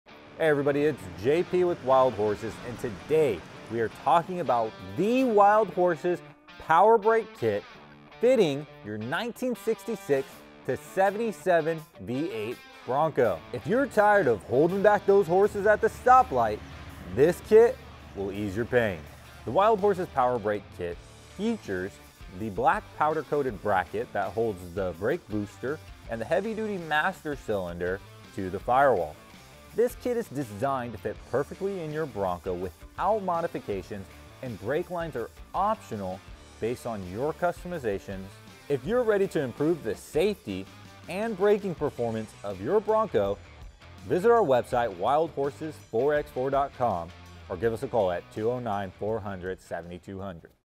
WILD HORSES Power Brake Kit
So you're tired of holding back those horses at the stop light. This kit will help ease your pain. With the WH kit there is no need to modify the inner fender. The brand new, dual diaphragm, booster fits right in place. Our black powder coated bracket holds the booster and our new heavy duty master cylinder (#3079) to the fire wall. Brake lines are optional because of all the custom things you might be doing such as brake lock etc.